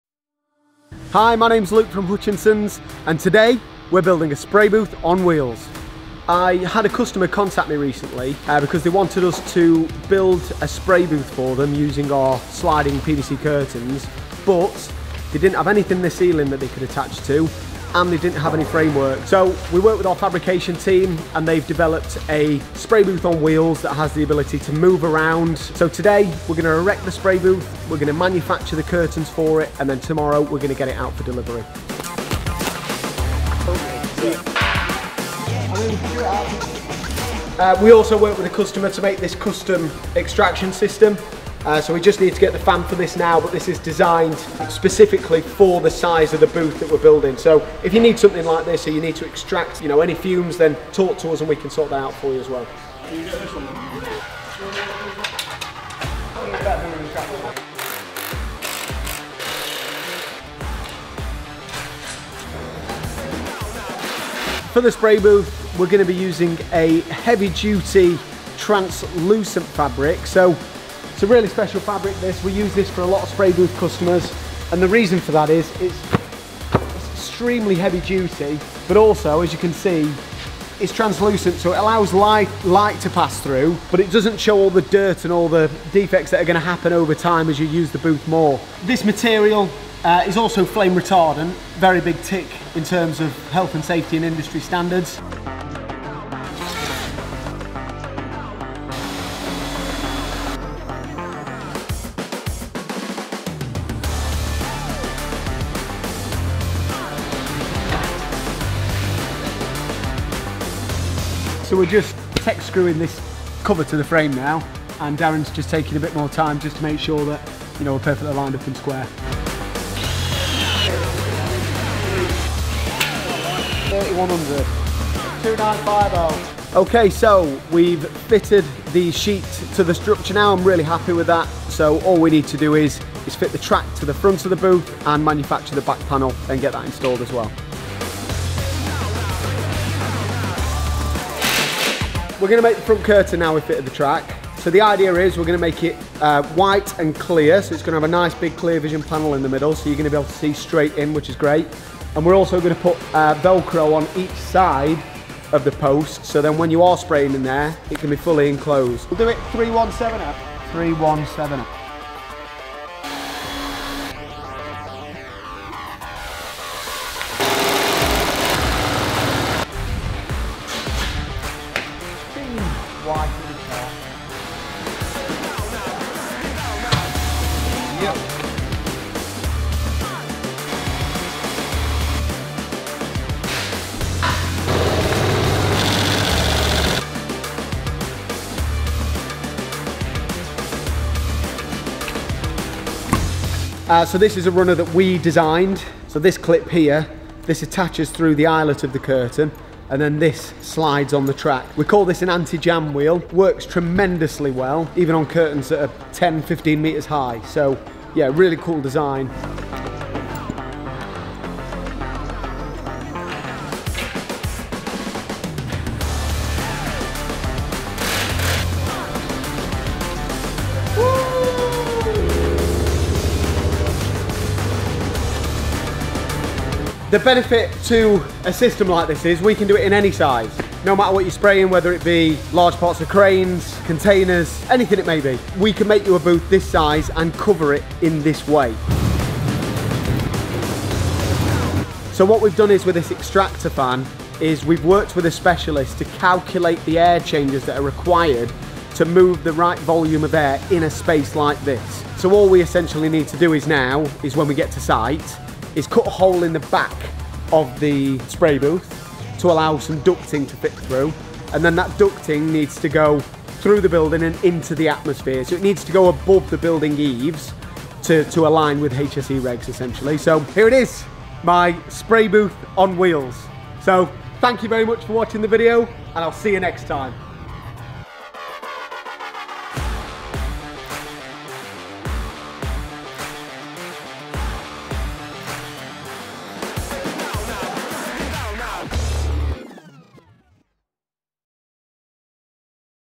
WE BUILT A SPRAY BOOTH ON WHEELS | PORTABLE SPRAY BOOTH PROJECT
Welcome to Hutchinson's PVC Solutions! In this video, we take you behind the scenes as we manufacture and install a state-of-the-art portable spray booth. This portable spray booth, designed with a robust box section frame and equipped with wheels, offers unmatched mobility and flexibility for all your industrial spray needs.

Watch as our skilled team expertly crafts this portable spray booth, ensuring top-quality construction and functionality. We’ll walk you through each step of the process, from initial assembly to final installation, demonstrating the ease and efficiency of setting up this versatile portable spray booth in any location.

Our portable spray booth is perfect for various applications, providing a controlled environment for painting, coating, and finishing tasks. Its mobility allows you to move it wherever needed, making it an ideal solution for dynamic and demanding workspaces.

Don't miss out on this in-depth look at our innovative portable spray booth.